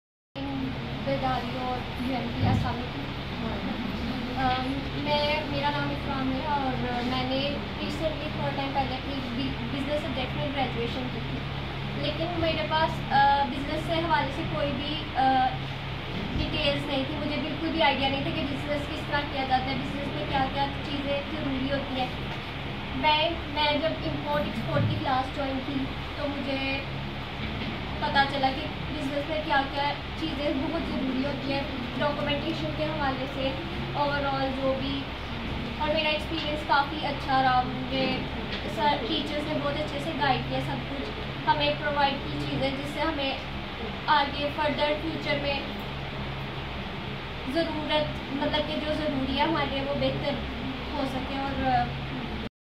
Baidarie Sialkot: A trainee addressing the participants of the certifcation ceremony.
20 Feb 2023: A trainee sharing her feelings and impressions about the utility of her training she received. She was addressing the participants of the certification ceremony organized for a batch of 75 beneficiaries of UNWomen supported WEE project who with the facilitation provided by Baidarie Sialkot became trained at University of Management & Technology Sialkot campus. Training was imparted to 25 women on import & export documentation, 25 women on office management and 25 on digital free lancing.